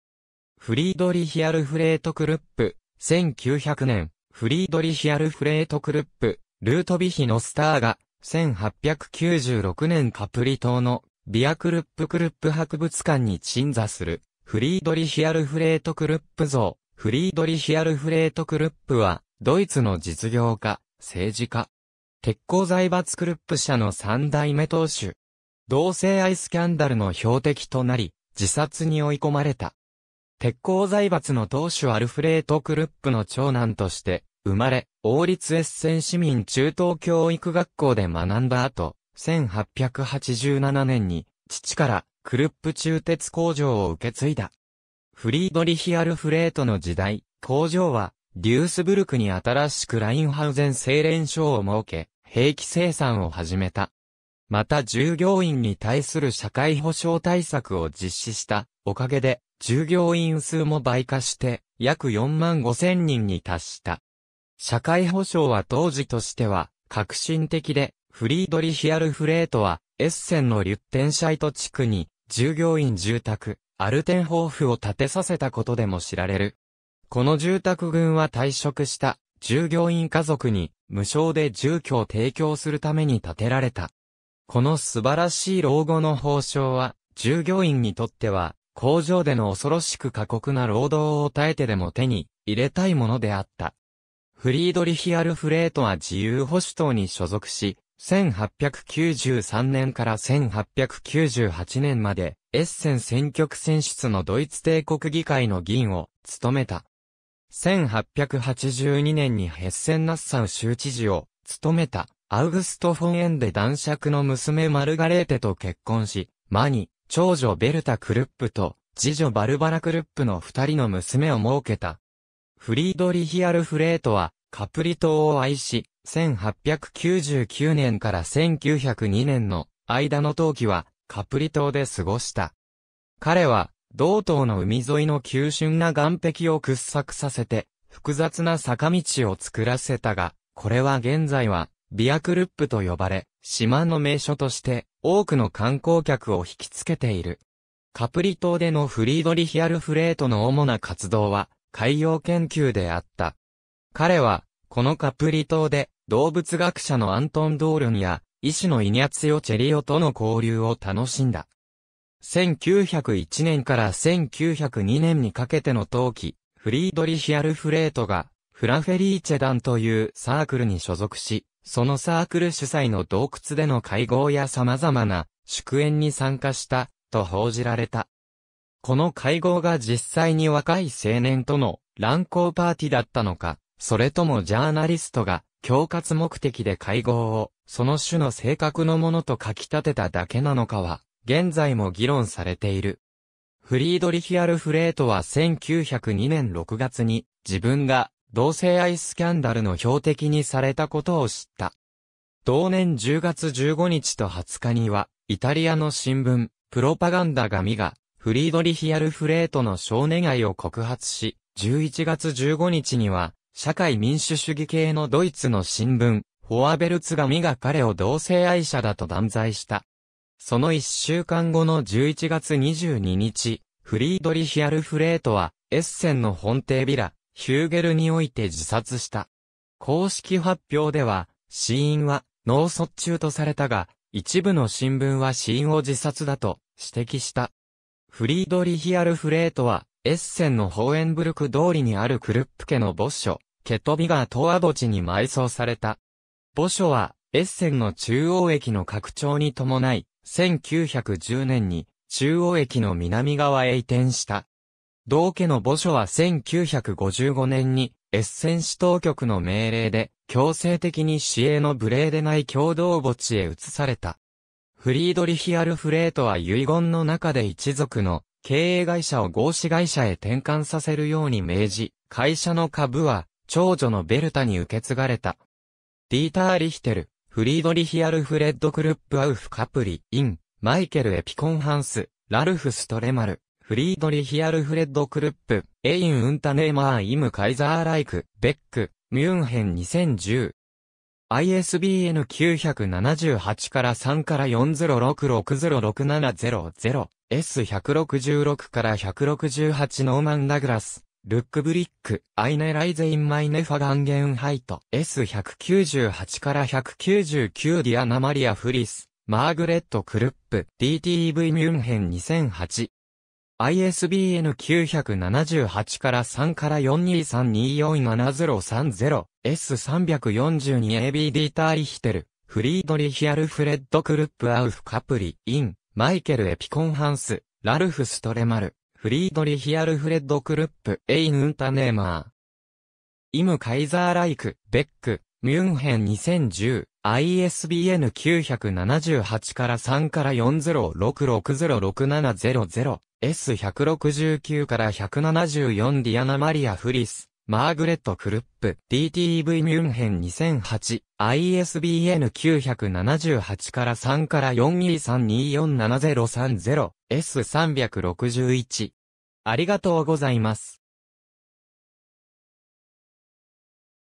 フリードリヒ・アルフレート・クルップ
フリードリヒ・アルフレート・クルップ、1900年 フリードリヒ・アルフレート・クルップ、ルートヴィヒ・ノスター画、1896年 カプリ島のヴィア・クルップ クルップ博物館（もとヴィラ・ヒューゲルの一部）に鎮座するフリードリヒ・アルフレート・クルップ像 フリードリヒ・アルフレート・クルップ（Friedrich Alfred Krupp, 1854年2月17日 エッセン - 1902年11月22日 エッセン）は、ドイツの実業家、政治家。
鉄鋼財閥クルップ社（ティッセンクルップ社の前身）の3代目当主。
同性愛スキャンダルの標的となり、自殺に追い込まれた。
鉄鋼財閥の当主アルフレート・クルップの長男として生まれ、王立エッセン市民中等教育学校（Königlichen Gymnasium Essen）で学んだ後、1887年に父からクルップ鋳鉄工場（Krupp Gussstahlfabrik）を受け継いだ。
フリードリヒ・アルフレートの時代、工場はデュースブルクに新しくラインハウゼン製錬所（de:Hütten- und Bergwerke Rheinhausen）を設け、兵器生産を始めた。
また従業員に対する社会保障対策を実施したおかげで、従業員数も倍加して約4万5000人に達した。
社会保障は当時としては革新的で、フリードリヒ・アルフレートはエッセンのリュッテンシャイト地区に従業員住宅アルテンホーフ（Altenhof）を建てさせたことでも知られる。
この住宅群は退職した従業員家族に無償で住居を提供するために建てられた。
この素晴らしい老後の報償は、従業員にとっては工場での恐ろしく過酷な労働を耐えてでも手に入れたいものであった。
フリードリヒ・アルフレートは自由保守党に所属し、1893年から1898年まで、エッセン選挙区選出のドイツ帝国議会の議員を務めた。
1882年にヘッセン＝ナッサウ州知事を務めたアウグスト・フォン・エンデ男爵（August von Ende）の娘マルガレーテと結婚し、間に長女ベルタ・クルップ（1886年 - 1957年）と次女バルバラ・クルップ（1887年 - 1972年）の2人の娘をもうけた。
フリードリヒ・アルフレートはカプリ島を愛し、1899年から1902年の間の冬季はカプリ島で過ごした。
彼は同島の海沿いの急峻な岩壁を掘削させて複雑な坂道を造らせたが、これは現在はヴィア・クルップと呼ばれ、島の名所として多くの観光客をひきつけている。
カプリ島でのフリードリヒ・アルフレートの主な活動は海洋研究であった。
彼はこのカプリ島で動物学者のアントン・ドールンや医師のイニャツィオ・チェリオとの交流を楽しんだ。
1901年から1902年にかけての冬季、フリードリヒ・アルフレートが「フラ・フェリーチェ団（Congrega di Fra Felice）」というサークルに所属し、そのサークル主催の洞窟での会合や様々な祝宴に参加した、と報じられた。
この会合が実際に若い青年との乱交パーティだったのか、それともジャーナリストが恐喝目的で会合をその種の性格のものと書き立てただけなのかは、現在も議論されている。
フリードリヒ・アルフレートは1902年6月に自分が同性愛スキャンダルの標的にされたことを知った。
同年10月15日と20日にはイタリアの新聞「プロパガンダ（Propaganda）」紙がフリードリヒ・アルフレートの少年愛を告発し、11月15日には社会民主主義系のドイツの新聞「フォアヴェルツ」紙が彼を同性愛者だと断罪した。
その1週間後の11月22日、フリードリヒ・アルフレートはエッセンの本邸ヴィラ・ヒューゲルにおいて自殺した。
公式発表では死因は脳卒中とされたが、一部の新聞は死因を自殺だと指摘した。
フリードリヒ・アルフレートはエッセンのホーエンブルク通りにあるクルップ家の墓所ケトヴィガー・トーア墓地（Friedhof am Kettwiger Tor）に埋葬された。
墓所はエッセンの中央駅の拡張に伴い、1910年に中央駅の南側へ移転した。
同家の墓所は1955年にエッセン市当局の命令で強制的に市営のブレーデナイ共同墓地（Friedhof Bredeney）へ移された。
フリードリヒ・アルフレートは遺言の中で一族の経営会社を合資会社へ転換させるように命じ、会社の株は長...